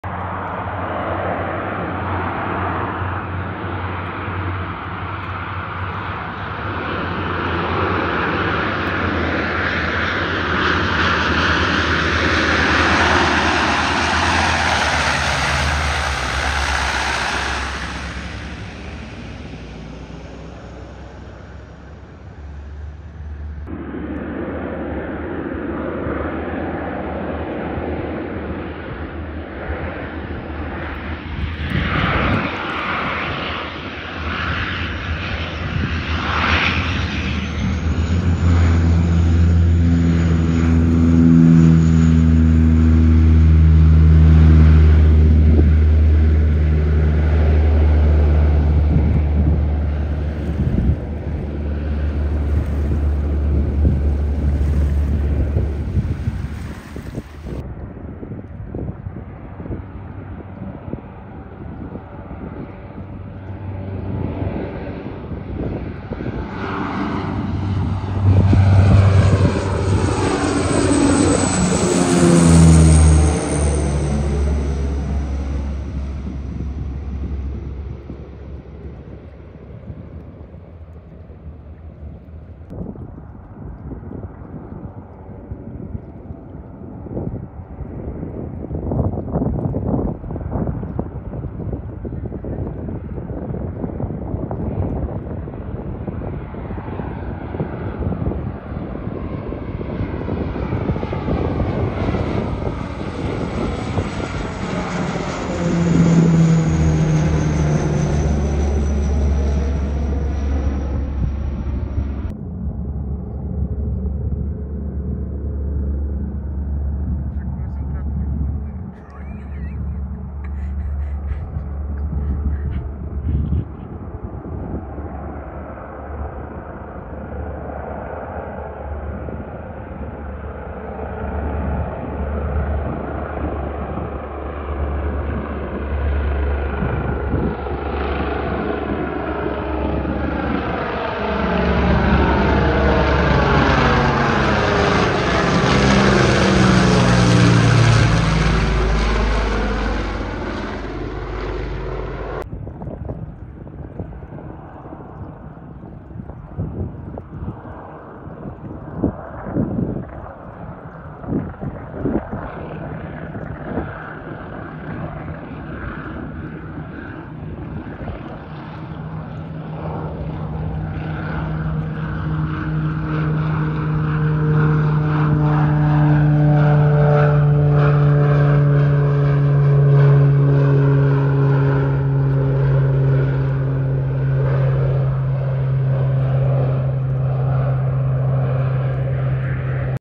Canadair & Air Tractor at Vilar Dam, Portugal - 18.09.2024 - 4K
Canadair & Air Tractor collecting water for aerial firefighting - Vilar Dam, Portugal - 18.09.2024